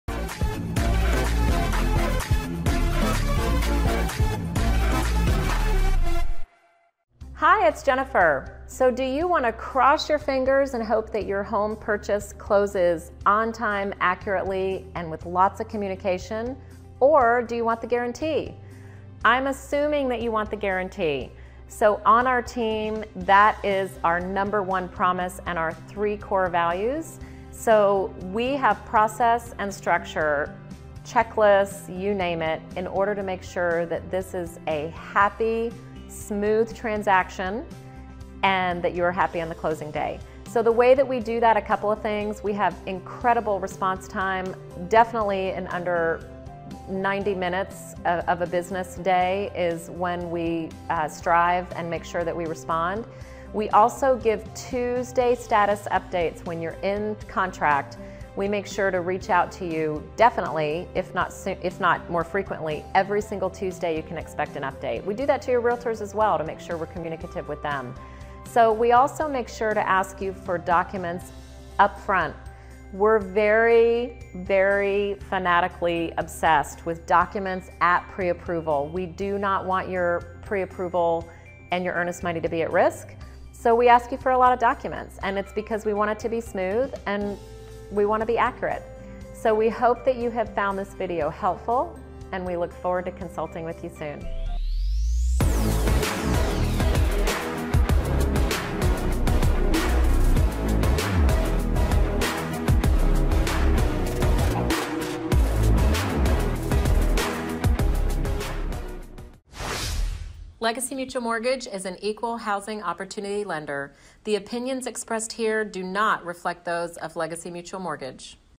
Don't Make this Mistake and Lose your House While in Escrow
Do you want to cross your fingers and hope that your home purchase closes on time?
The lender you choose can make all the difference.
Make sure you have lots of communication!

To get the guarantee closing on time, you need to choose the right lender.

✅ Watch now to make sure you get the house you want!

CHAPTERS:
0:00 Introduction
0:15 Process for a stress free closing
0:40 Response time from your lender
0:50 weekly updates from your lender
1:10 Docs required at the time of application

Jennifer Hughes Hernandez, Legacy Mutual Mortgage - Equal Housing Opportunity Lender NMLS #514497